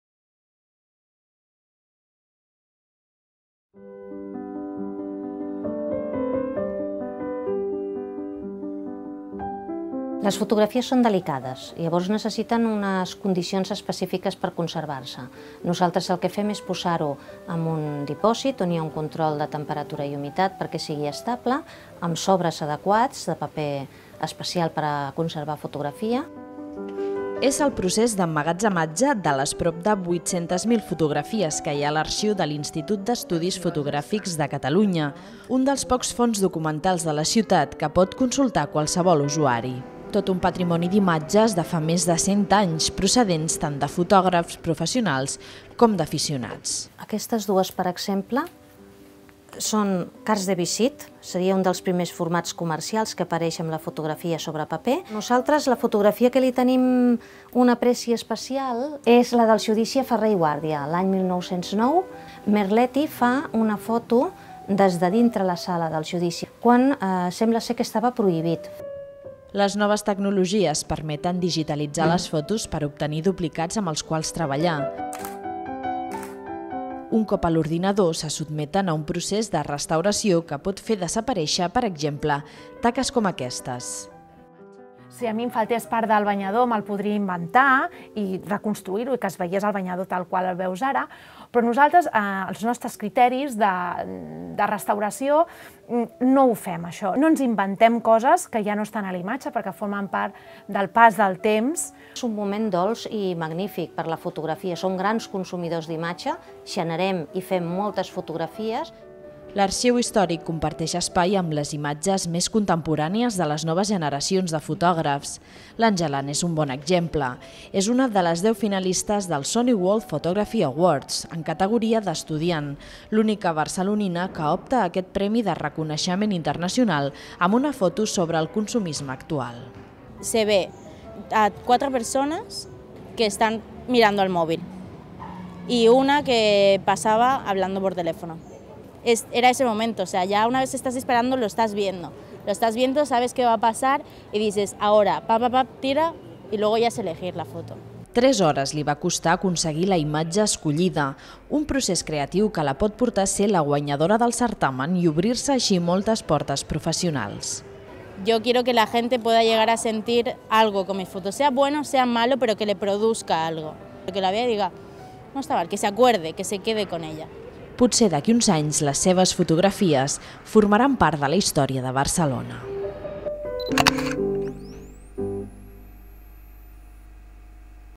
Passat i present de la fotografia a l'Institut d'Estudis Fotogràfics de Catalunya
L'arxiu fotogràfic de l'Institut d'Estudis Fotogràfics de Catalunya fa 40 anys que funciona i té 800.000 fotos antigues. Més informació